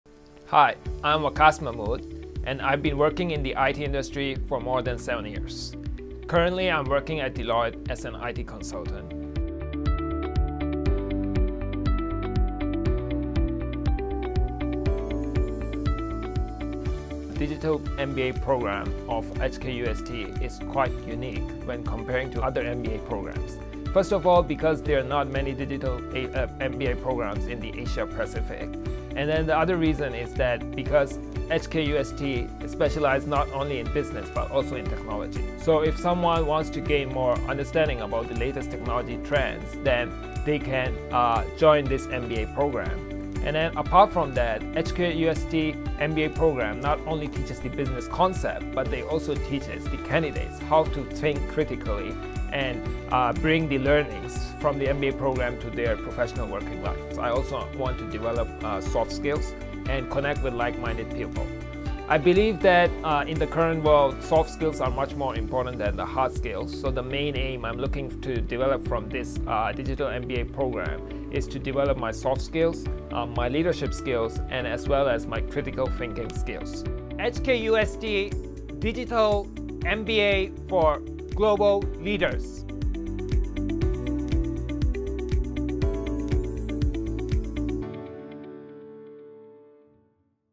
HKUST Digital MBA Student Sharing - Waqas Mahmood
Waqas Mahmood (DiMBA Intake 2023) choose the HKUST Digital MBA (DiMBA) program because of its unique blend of business and technology education. The program stands out in the Asia-Pacific region, integrating the latest technology trends with robust business concepts, making it ideal for professionals like Waqas who want to stay ahead in the tech-driven world.